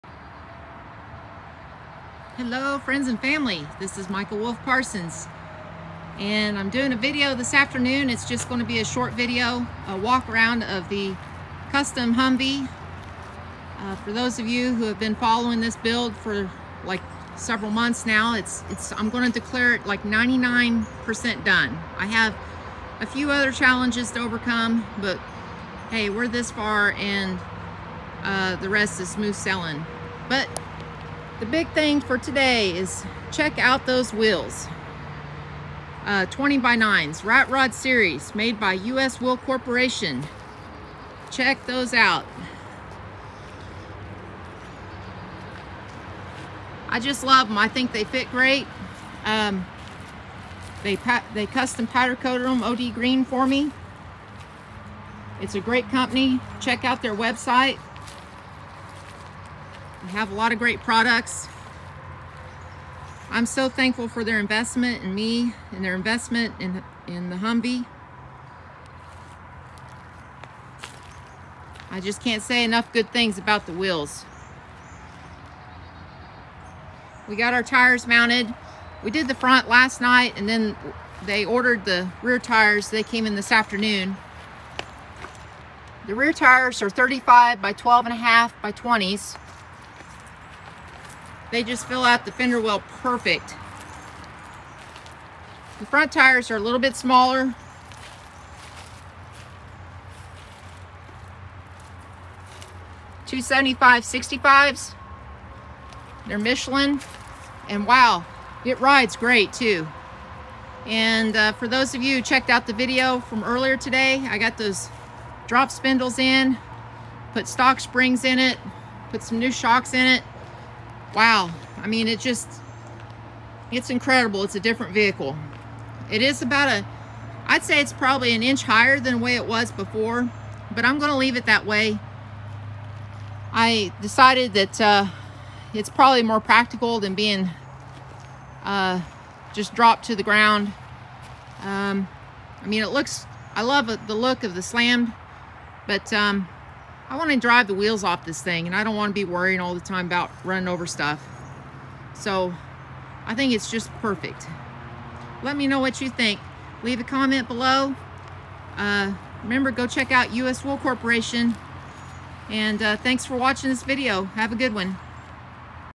Custom HMMWV with 20x9 U.S. Wheel Corporation wheels and Michelin tires!
Check this out! Our U.S. Wheel Corporation wheels have finally arrived and have been installed on our custom HMMWV. We are running Michelin tires, michelintires!